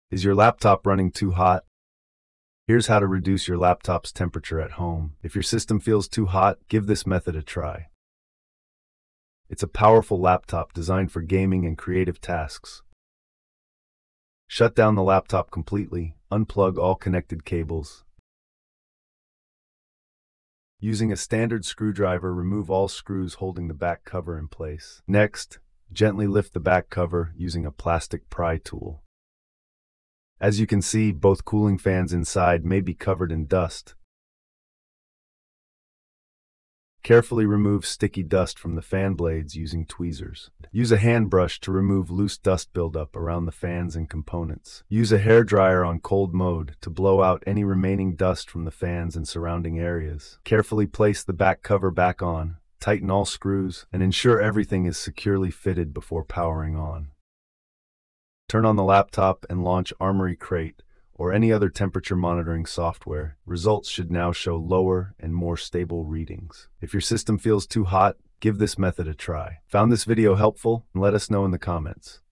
FIX OVERHEATING FAST! 🔥 NO TOOLS NEEDED (ASUS TUF A15 Tutorial)
📌 Description:
Laptop overheating while gaming or editing? In this step-by-step ASUS TUF Gaming A15 cleaning tutorial, learn how to fix high CPU & GPU temperatures (80–90°C) at home—without any special tools! Using simple items like a brush, hair dryer (cold mode), and plastic pry tool, you can reduce heat buildup caused by dust and improve your laptop's performance.

This guide is perfect for ASUS TUF A15 2023 users with the AMD Ryzen 9 7940HS and RTX 4070 Laptop GPU, but the same method applies to many gaming laptops. Watch till the end for before-and-after temps!

✅ Steps Covered in This Video:

Temperature check with Armoury Crate

Back cover removal

Fan dust cleaning (brush + tweezers)

Safe use of hair dryer (cold mode)

Keyboard and screen cleaning

Reassembly and temperature results